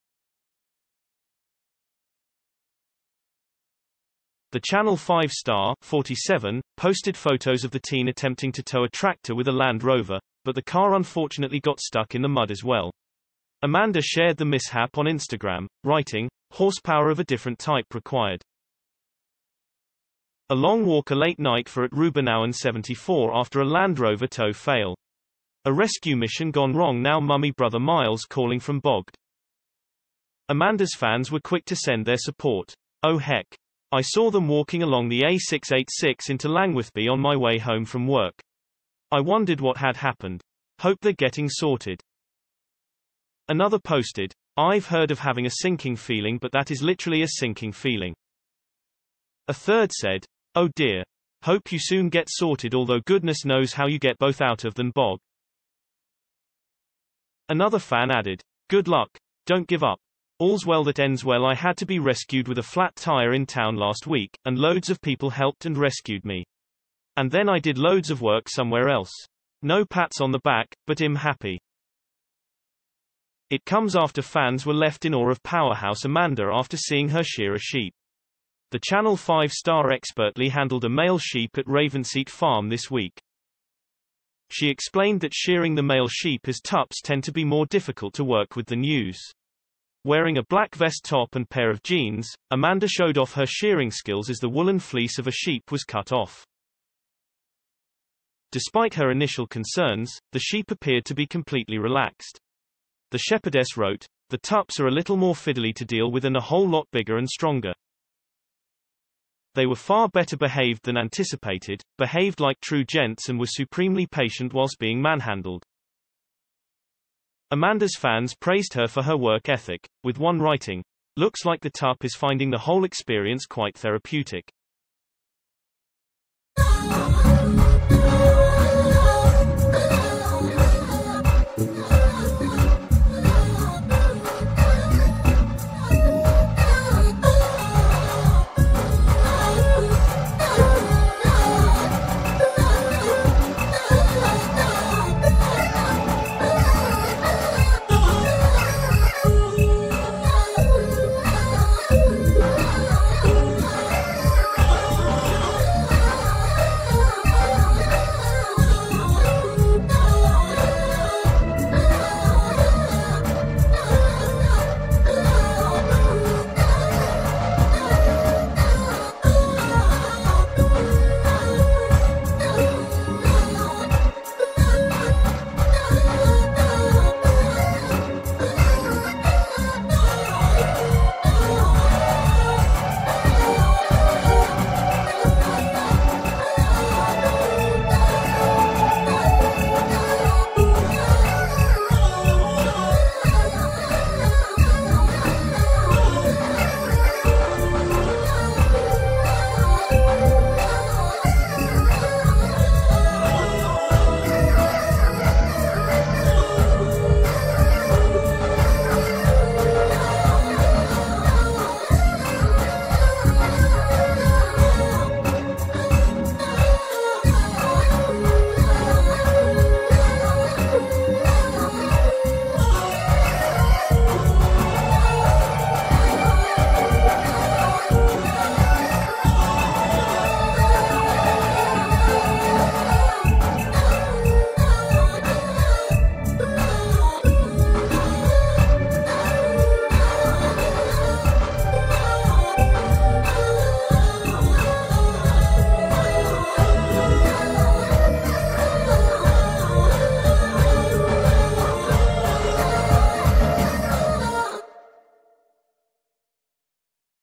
Our Yorkshire Farm fans flood Amanda Owen with support after ‘serious’ farming mishap
OUR Yorkshire Farm fans rushed to support Amanda Owen's son Reuben after a failed "rescue mission".